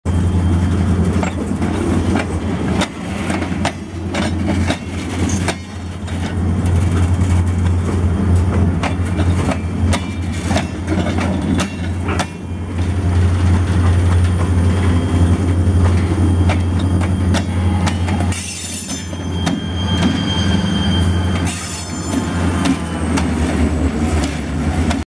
Missing Chunk of Train Track on Active Rail | Jason Asselin
VISIT MY ETSY STORE, SEE WHAT I HAVE FOR SALE:
Everyday a train passes by our house in East Kingsford, MI. Seeing this chunk of track really makes me wonder how bad these rails really are. I am expecting a train to derail here soon by what I see here.